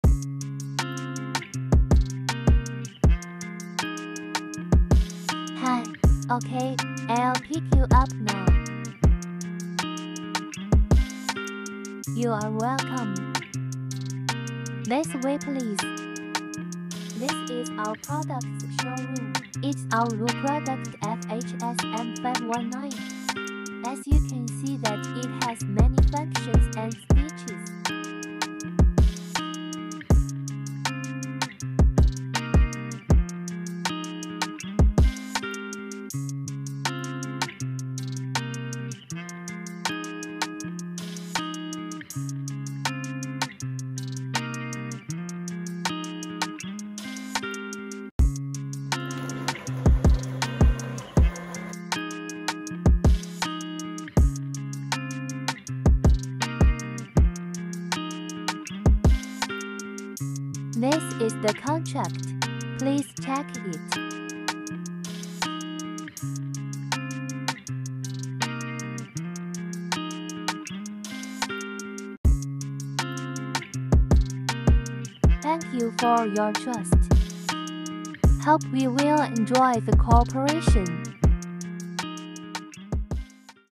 VOF Household Sewing Machine Creative Video: Receiving Customers
Huafeng mechanical & electrical co.,ltd is an international sewing machine manufacturer. We receive orders from all over the world every day, some customers trade online, and some choose to submit online. So today we will simulate the offline face-to-face meeting process between us and our customers. Let everyone know our Huafeng hospitality and our excellent sewing machine products